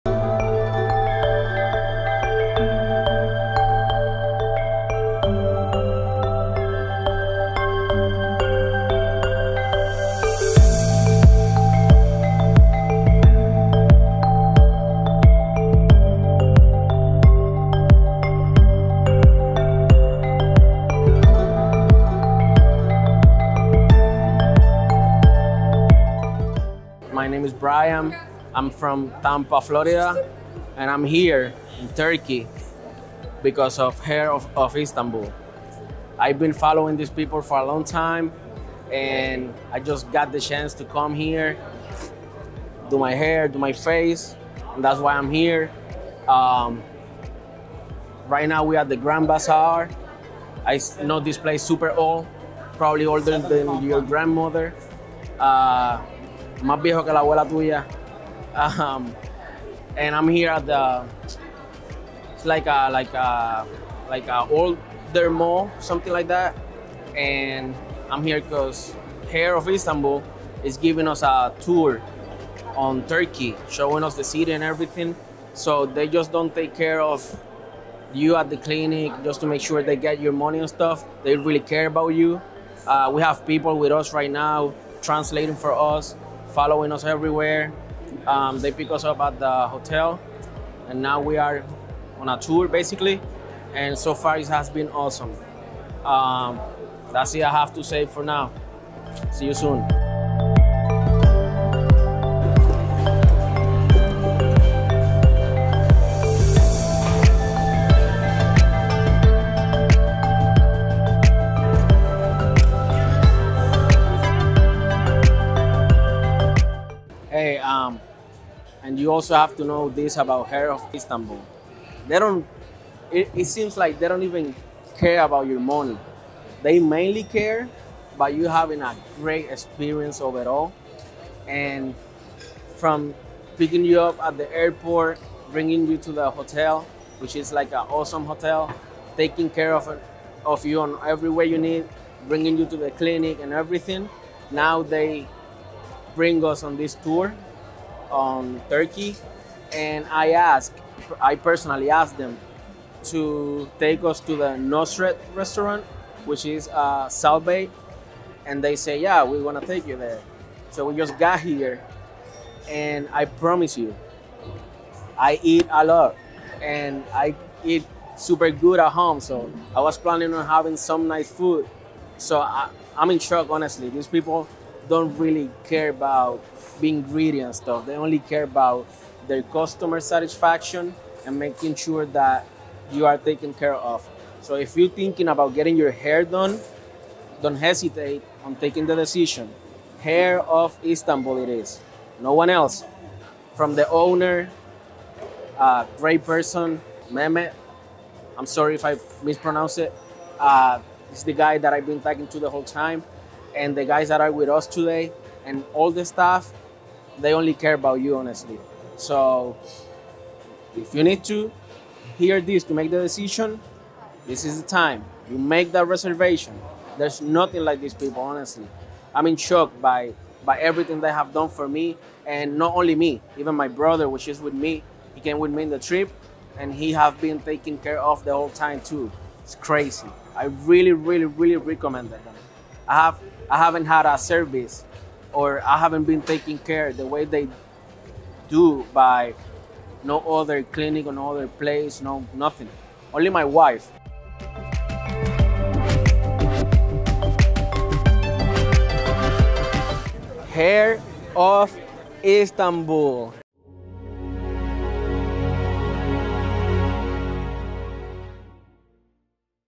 After Hair and Beard Transplant Surgery | Istanbul Grand Bazaar Tour
This video contains our patient Brian's trip to the Grand Bazaar and Istanbul tour after the hair and beard transplantation...

Our patient, who visited the historical places of Istanbul after hair and beard transplantation, wanted to go for dinner at Salt Bae (Nusr-et) restaurant, and we took him. He was planning to have delicious meals for dinner.

In this video, You can watch our patient's opinions and experiences about hair and beard transplantation, as well as other Hair of Istanbul services.